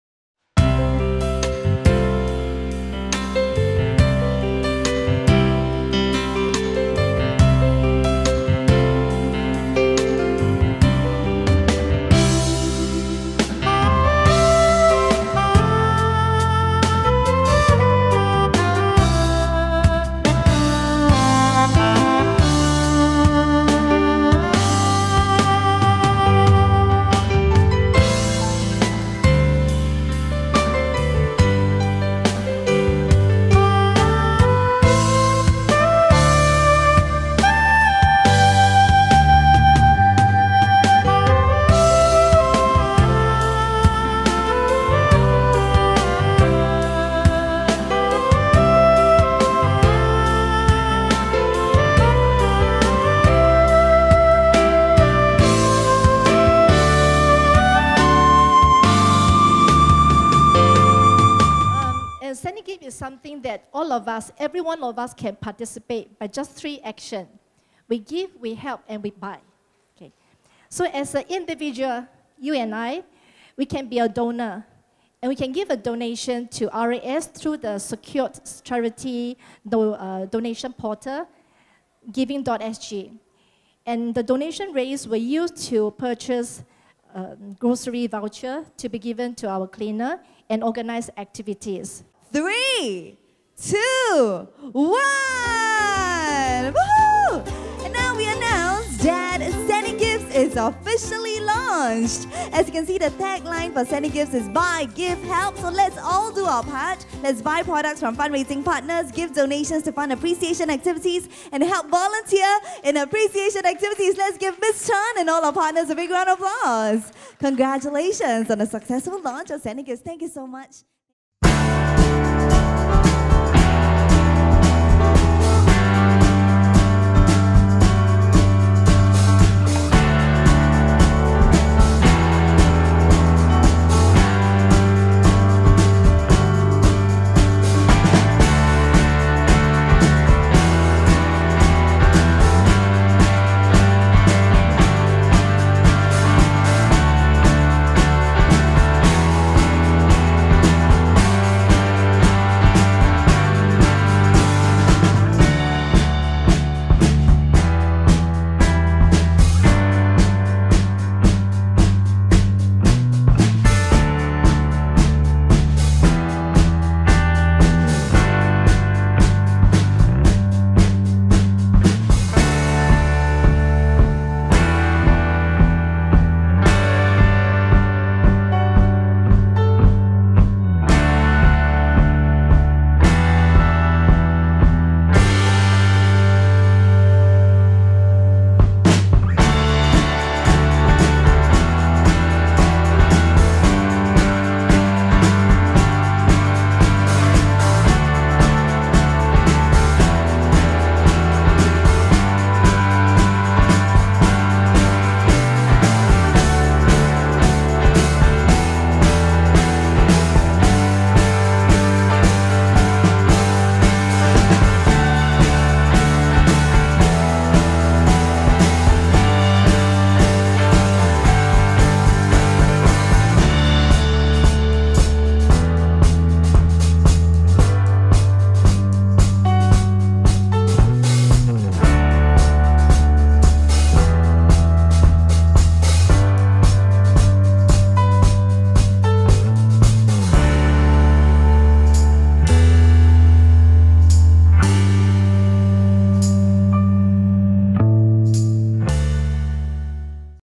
LOO (Let's Observe Ourselves) Awards 2017
The LOO Awards recognises any organisations and individuals for their contributions. SaniGives was also launched to raise public awareness about showing kindness and recognition towards fellow cleaning attendants.